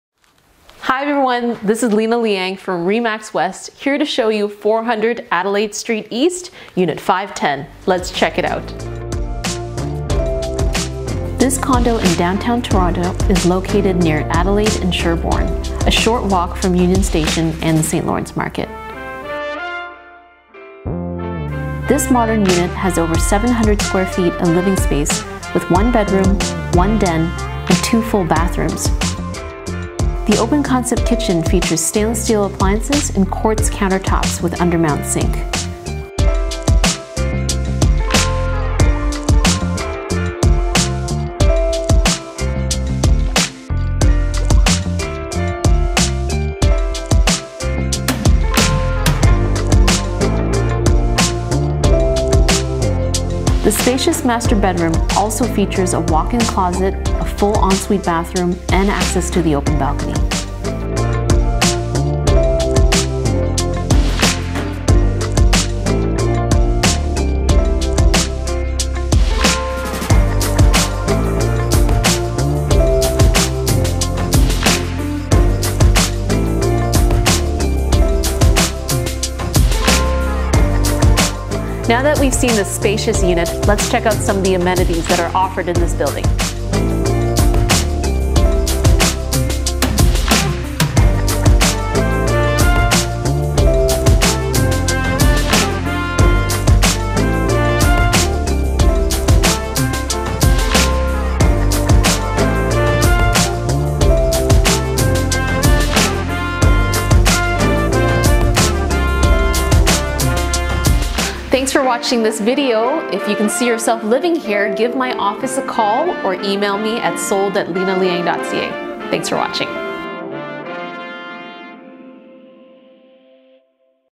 400 Adelaide Street East, Unit 510 - Toronto, Ontario (Walkthrough Tour)
Welcome to 400 Adelaide St E, Unit 510!
1+1 Bedroom, 2 Full Bathrooms
1 Parking, 1 Locker
Laminate Floors throughout
Over 700 Sqft of living space, plus an open balcony
Balcony has two access points (one from the living room, the other from the master bedroom)
Master Bedroom features a walk-in closet and a 4pc ensuite bathroom.
Kitchen has quartz countertops with undermount sink
Walkscore is 98, Transit score is 100